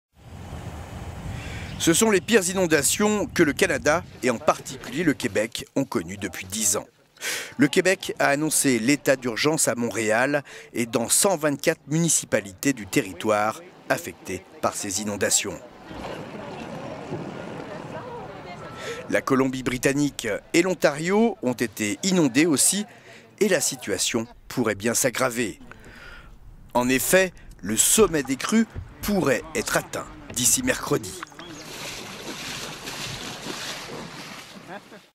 Inondations monstres au Québec
Ce sont les pires inondations que le Canada, et en particulier le Québec ont connu depuis 10 ans.

[Crue des eaux] Digues et murets déployés aux endroits névralgiques inondations. Pour obtenir des sacs de sable, communiquez avec le 311. Montréal Alertes (@MTL_Alertes) 8 mai 2017

Le Québec a annoncé l'état d'urgence à Montréal et dans 124 municipalités du territoire affectées par ces inondations.

  Autre journée difficile pour des milliers de québécois en rais…